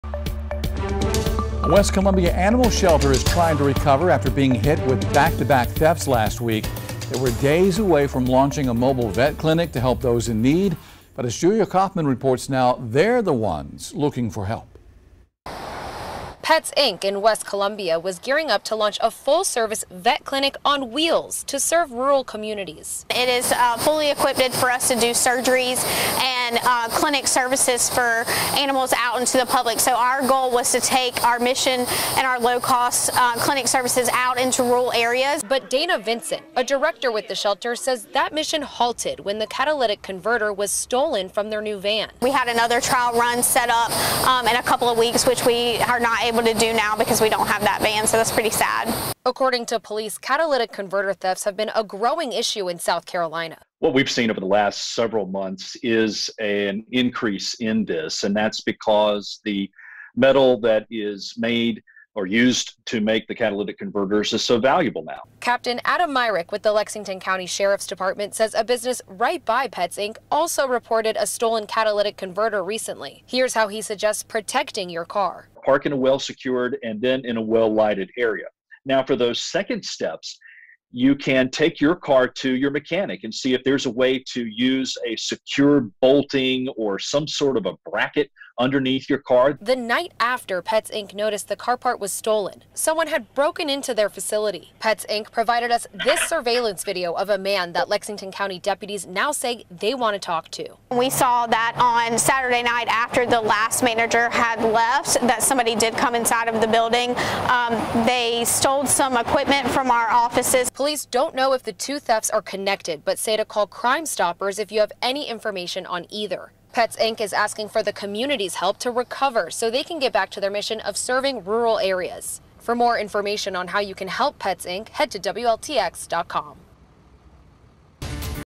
Midlands Animal shelter suffers back-to-back thefts, asking community for help
PETSinc in West Columbia was ready to launch their new mobile veterinarian clinic when the catalytic converter was stolen off their van. Then, there was a break-in.